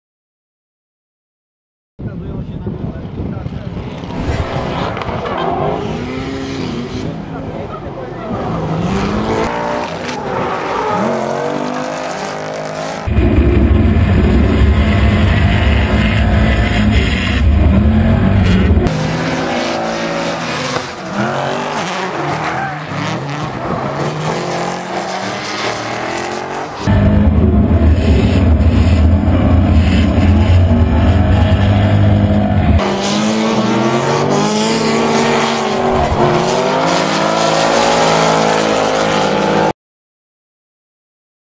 Drift.ro Shorts: LS BMW E30 versus SR20VET Nissan RPS13
Steve Biaggioni taking on Marcin Mospinek at Tor Jastrzab in Poland! This was a pretty nice battle to say the least!